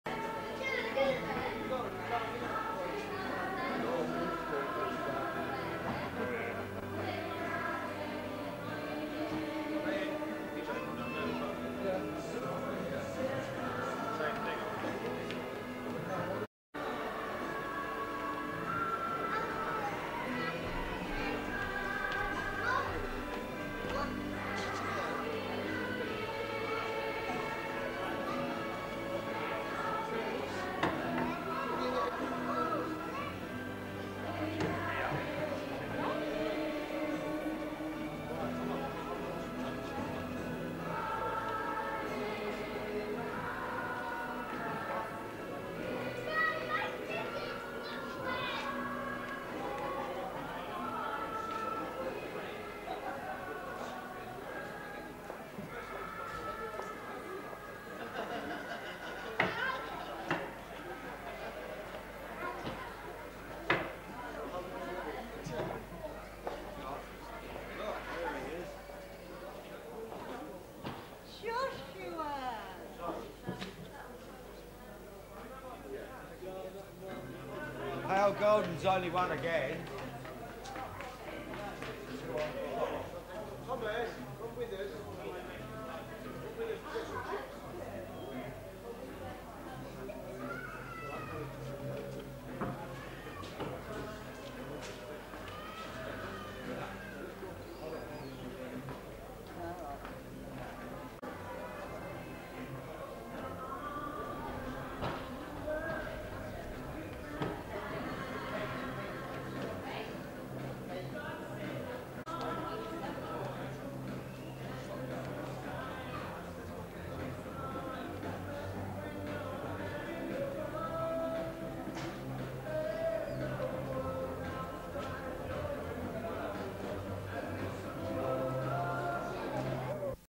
viola mist trap 1 wins his 2nd race on boxing day at coventry
viola mist wins his 2nd race at coventry on boxing day.